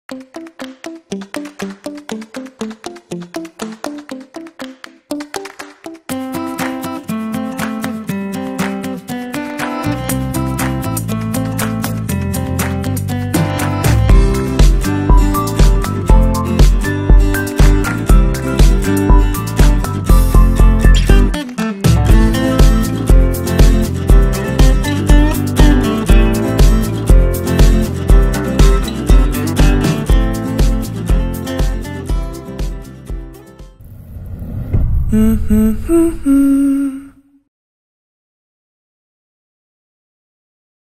Children's Arpenaz 50 Laces Trekking boots
Occasional Lowland Hiking In Dry Weather. First Technical Price, Robust And Lightweight. Rubber Sole For An Increased Life Span.